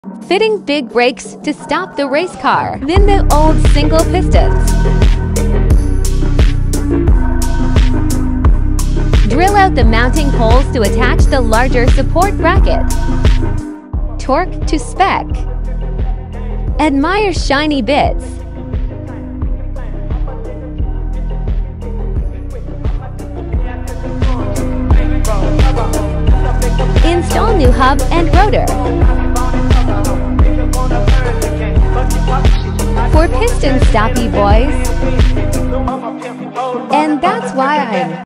How to make your car stop IMMEDIATELY
Installing wilwood 4 pistons on the XA falcon